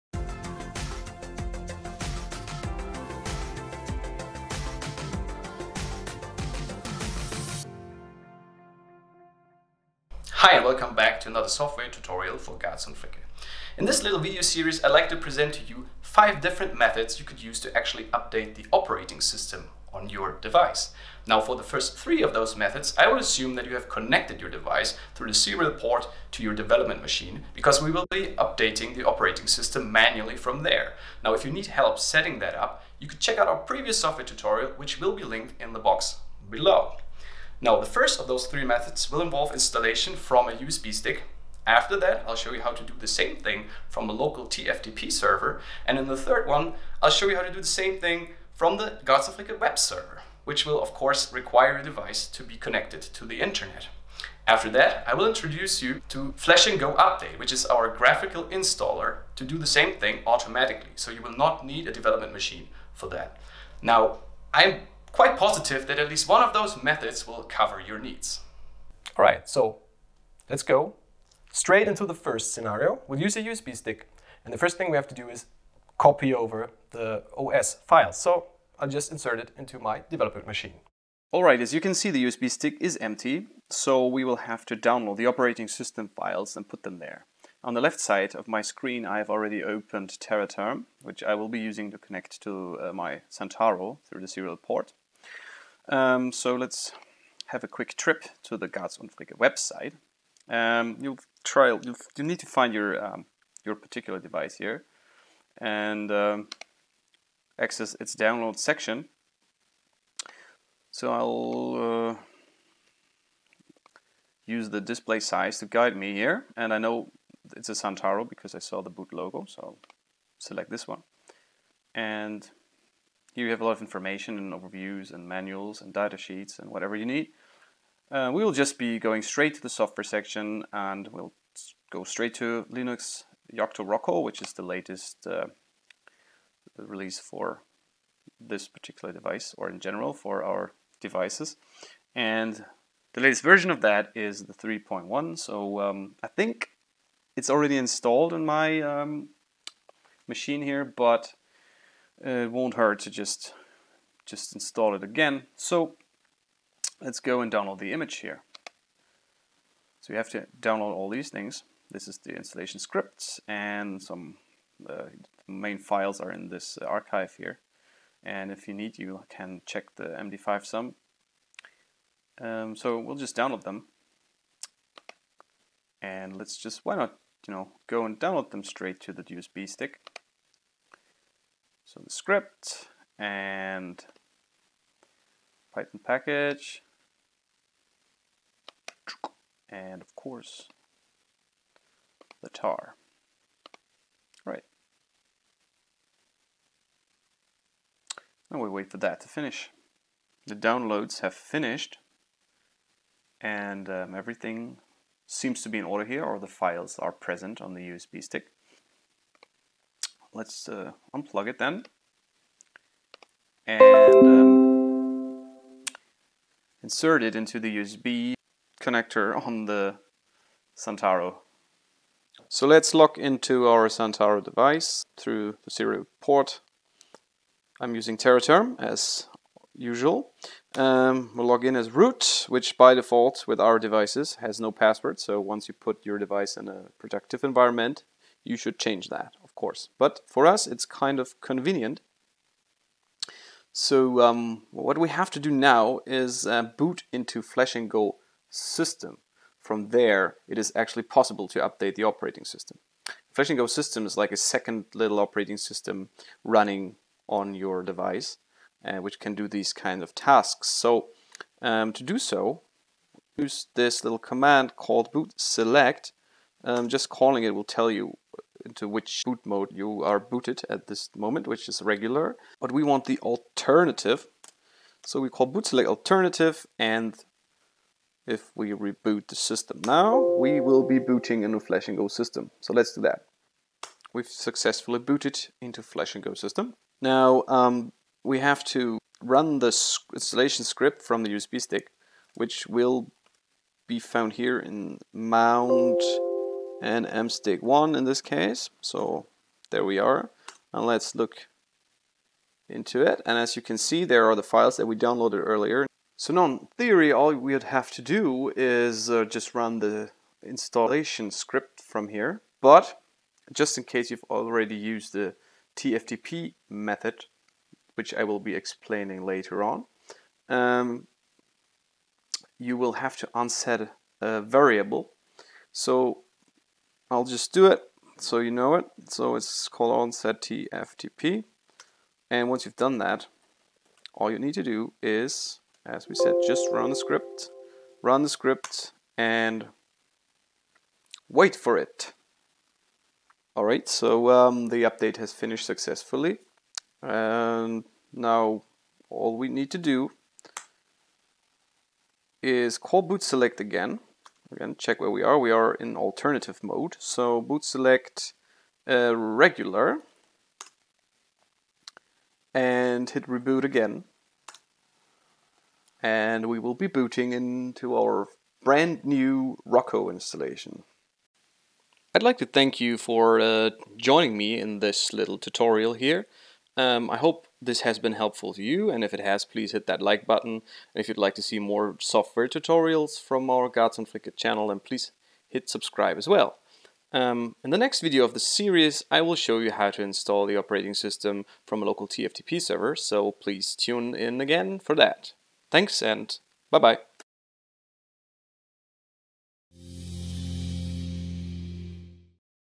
OS installation on your HMI using a USB stick (1/5)
This tutorial will show you how to update your Garz & Fricke HMI by using a USB stick.